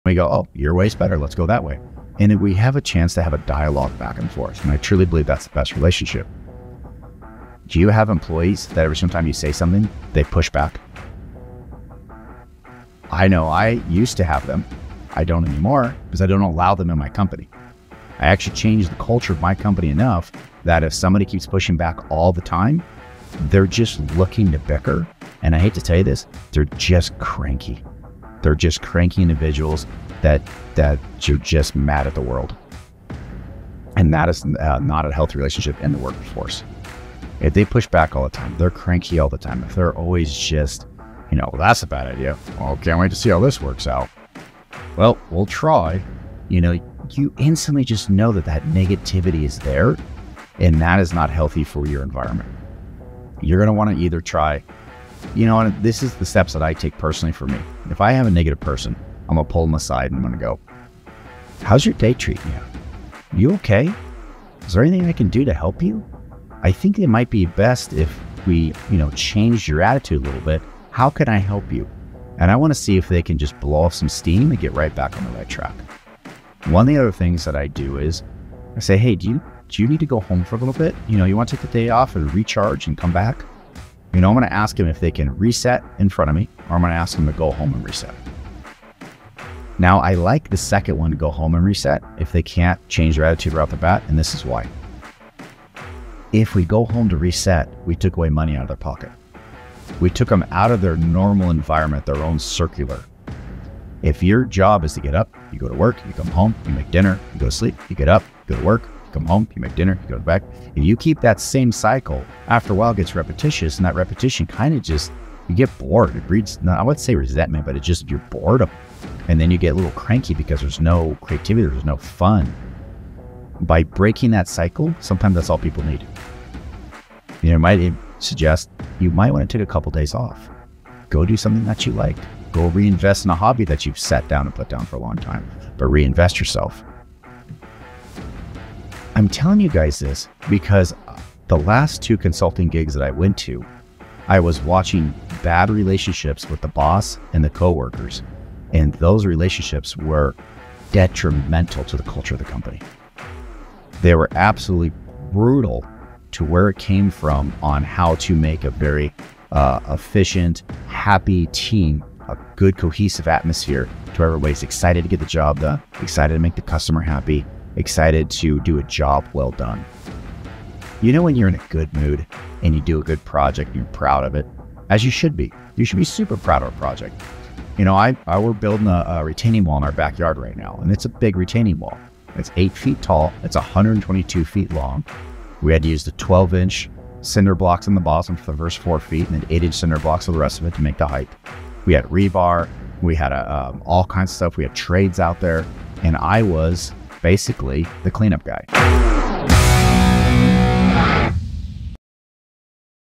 Nurturing Healthy Relationships: Creating a Positive Work Environment🤝🌱 | The AC Method Clips
In this new podcast episode, Aaron shares his insights on building strong relationships in the workplace that promote productivity, harmony, and personal growth. Discover the importance of open dialogue and how it can transform your team dynamics.

Have you encountered employees who push back when you present ideas? Aaron knows the feeling, but he made a significant change in his company's culture to eliminate constant negativity. Uncover the detrimental effects of cranky individuals and why fostering a healthy relationship is crucial for a thriving workforce. Learn effective strategies to handle negative team members. Aaron reveals his approach of addressing their concerns, offering support, and helping them reset their attitude. By encouraging individuals to take time off, recharge, and engage in activities they enjoy, you can break the monotony of their routine and reignite their passion.

Don't miss out on valuable tips and insights to build a successful and fulfilling career.

✅Get notified whenever Aaron goes posts!